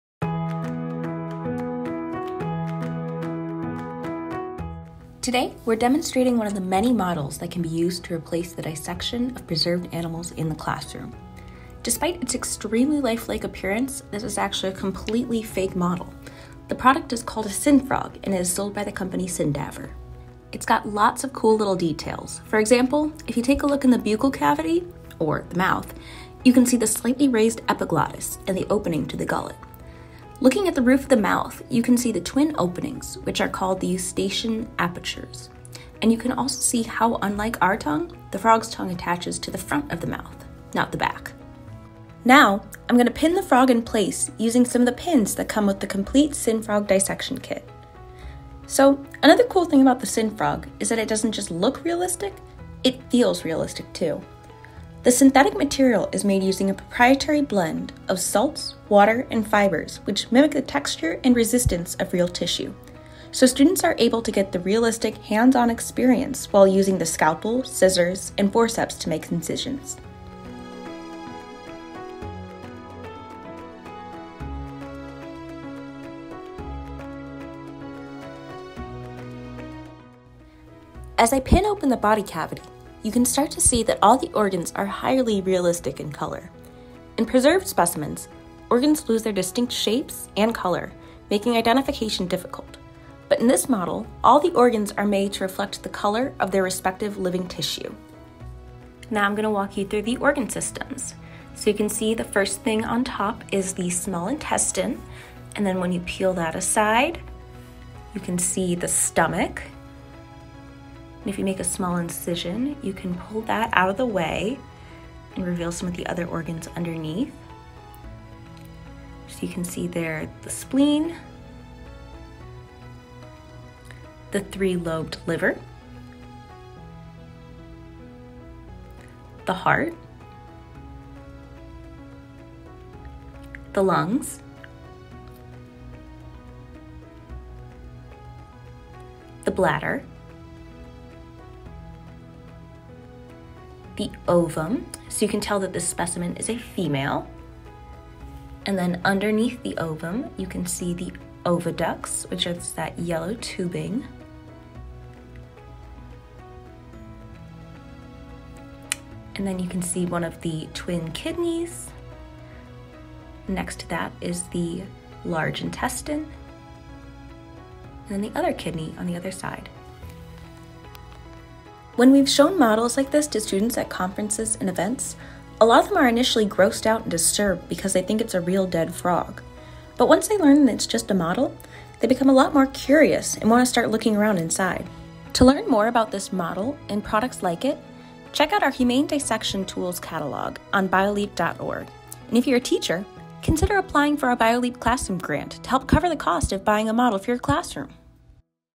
Humane Science Education Exploration - SynFrog Dissection
In this video, follow along as we take you through the process of dissecting a synthetic frog (SynFrog) created by the company SynDaver. This model is just one of many alternatives to dissection in the classroom.

To learn more about this model and others like it, please